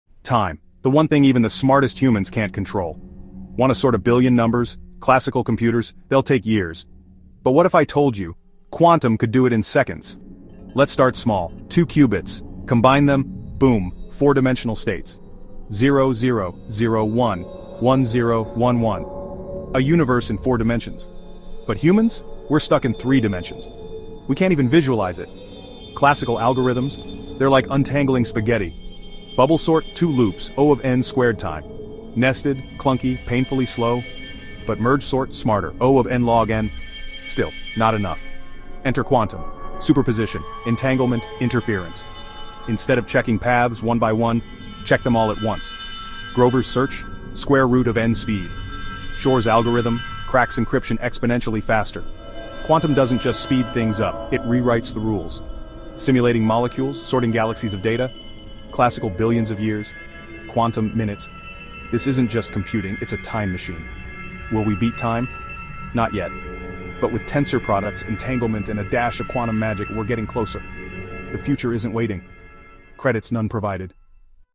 Quantum Computing vs. Time Itself
Dive into the fascinating world of quantum computing with our latest video, "Quantum Computing vs. Time Itself"! ⏳💻 Explore how quantum technology is set to revolutionize the way we process information—far beyond the limits of classical computers. Discover the magic of qubits, superposition, and entanglement in a visually stunning journey that showcases how quantum algorithms can tackle complex problems at lightning speed. From sorting billions of numbers to simulating molecules in minutes, this isn't just an evolution in computing—it's a leap toward mastering time itself!